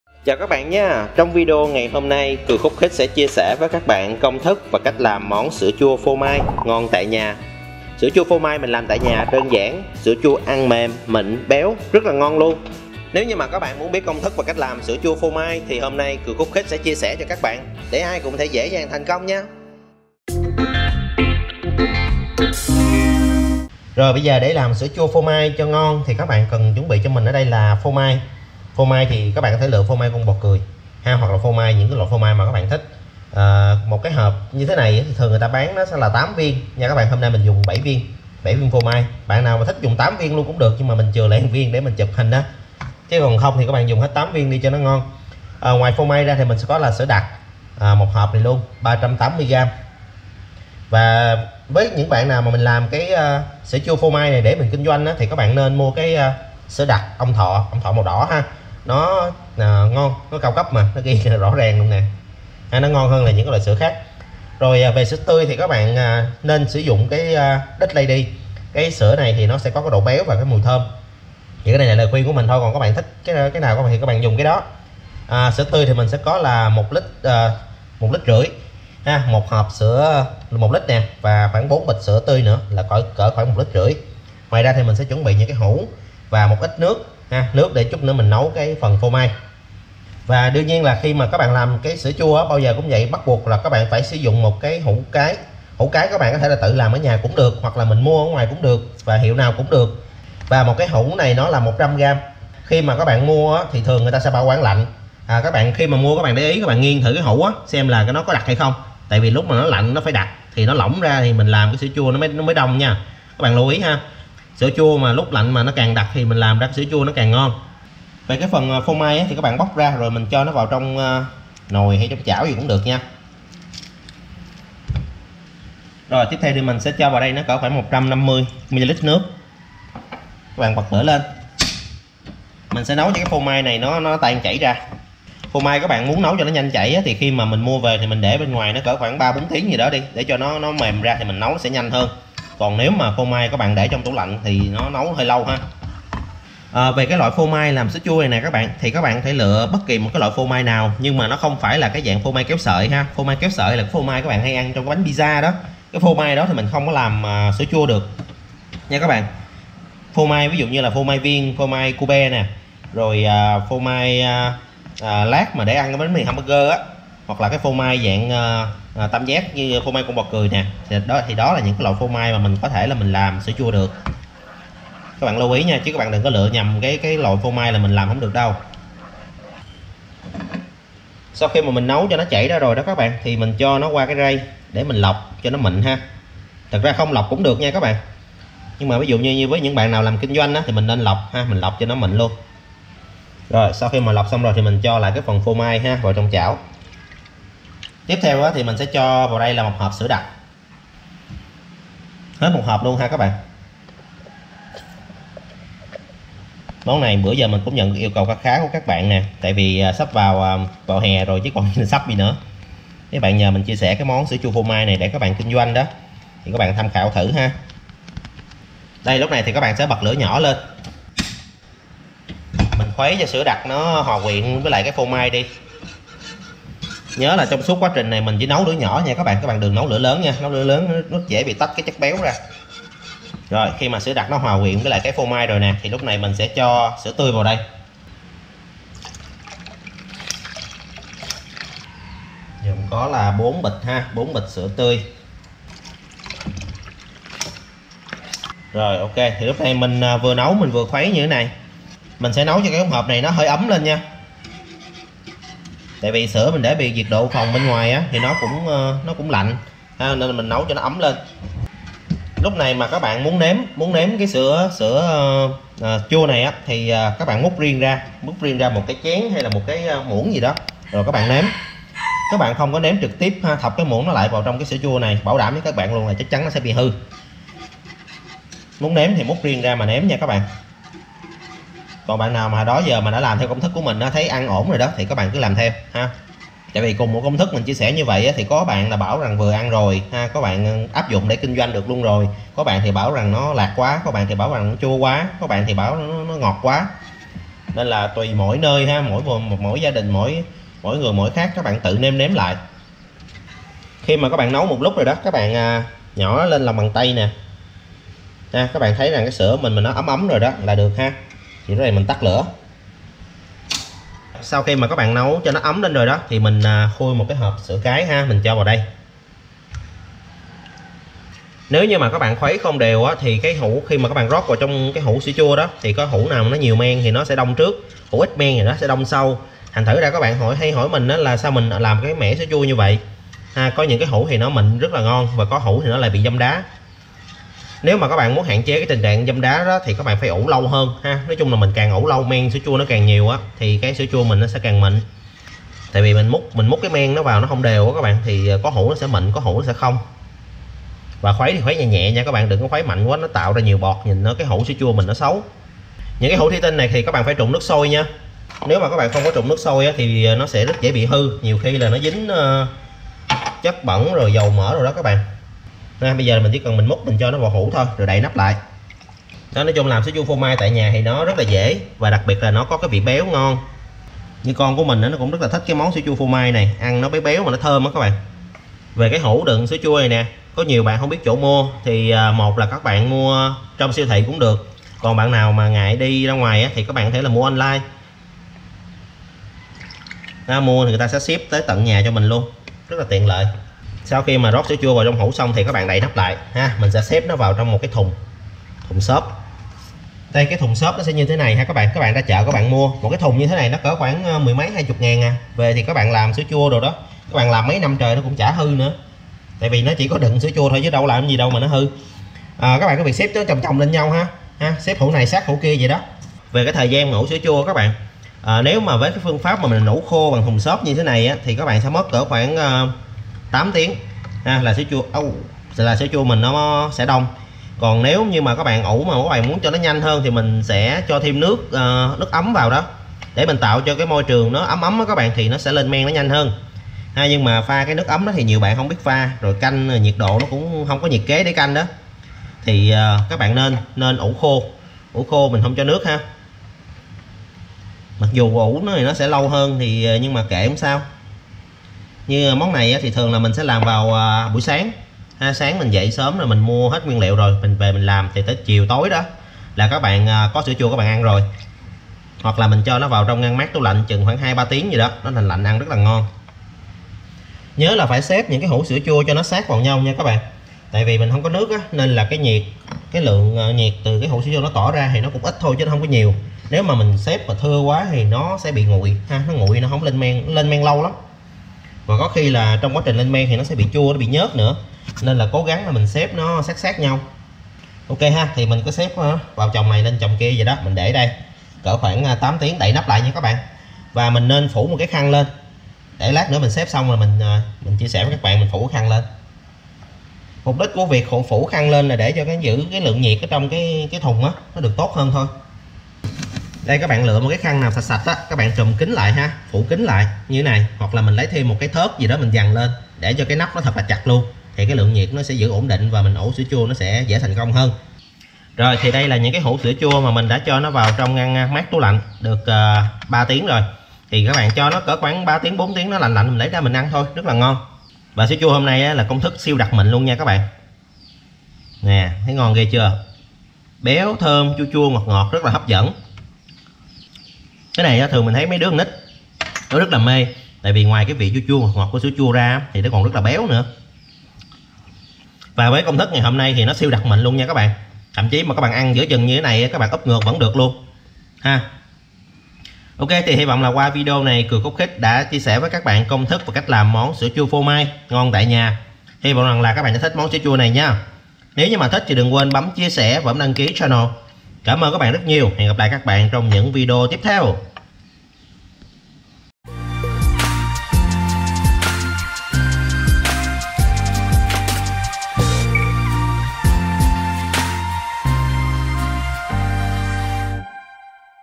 CÁCH LÀM SỮA CHUA PHÔ MAI Ủ KHÔNG CẦN NƯỚC SIÊU ĐẶC MỊN - CKK
Chia sẻ công thức và cách làm sữa chua phô mai tuyệt ngon tại nhà. Sữa chua phô mai ủ khô (ủ không cần nước) làm dễ thành công, sữa chua đặc mịn, béo thơm rất hấp dẫn.

♨ CÔNG THỨC SỮA CHUA PHÔ MAI NGON:
 - 1 hủ sữa chua cái
 - 1.88L sữa tươi
 - 7 viên phô mai (100g)
 - 380g sữa đặc
 - 25 hủ đựng sữa chua (loại 100g)
 - 150ml nước
 - thùng xốp

CKK chúc các bạn làm SữaChua SữaChuaPhôMai thành công! ⛄